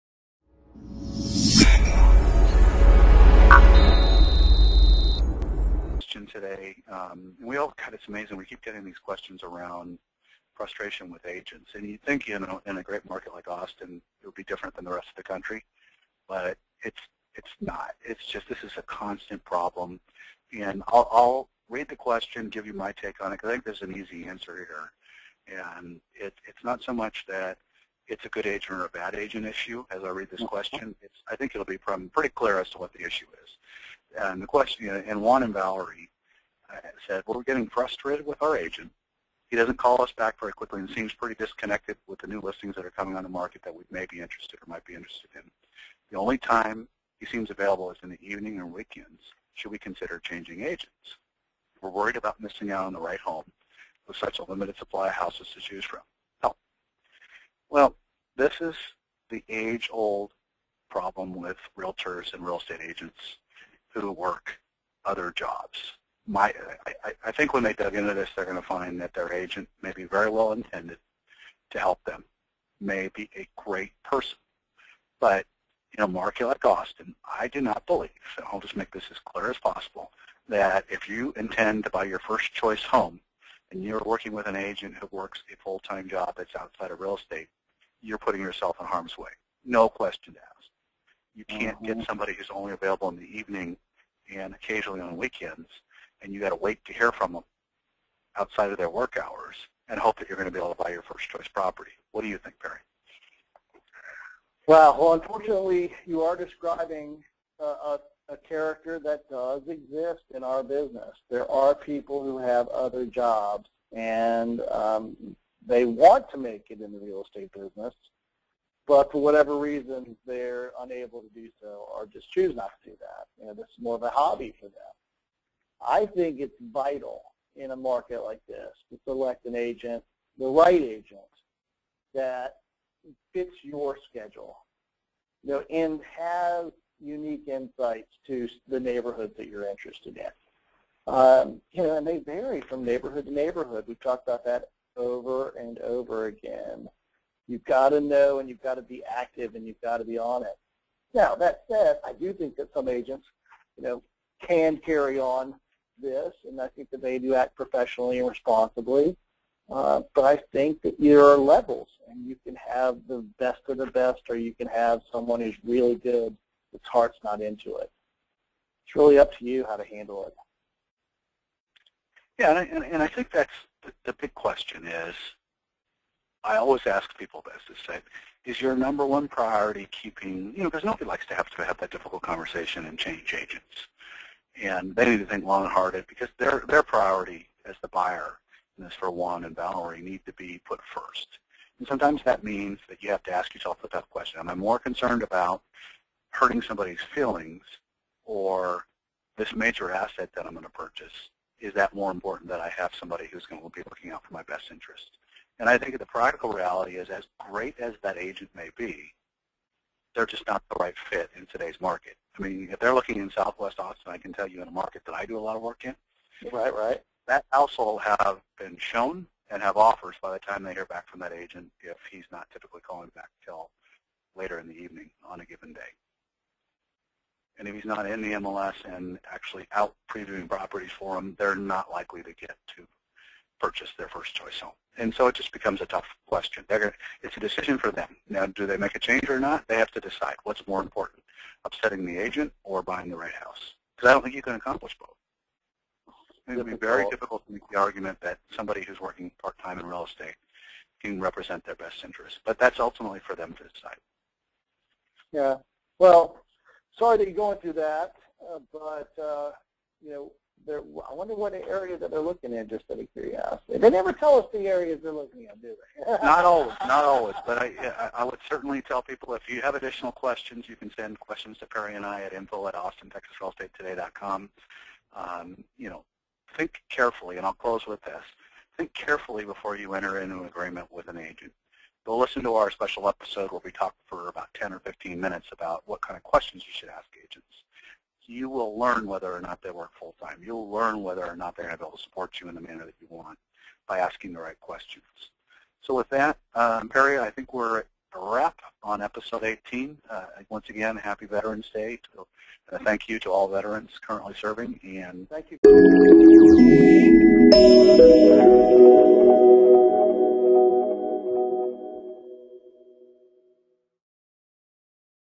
Austin Realty Podcast | Hiring the Right Austin Realtor | Full Time vs. Part Time with Tight Supply
Looking for Austin Realty or Austin Real Estate listings, be sure to visit Austin Real Estate Today.

Austin Top Realtors Kevin Cottrell from Re Max Austin Skyline and Perry Henderson from Prudential Texas Realty continue a discussion on an often asked topic from home buyers about how to ensure you're hiring the right Austin Realtor.

This weeks topic focuses on challenges in working with part-time vs. full-time Austin Real Estate Agents in the 2012 Austin Real Estate Market with the hosts offering their insight into how hiring the right agent (whether listing or buying) can make all the difference in your success.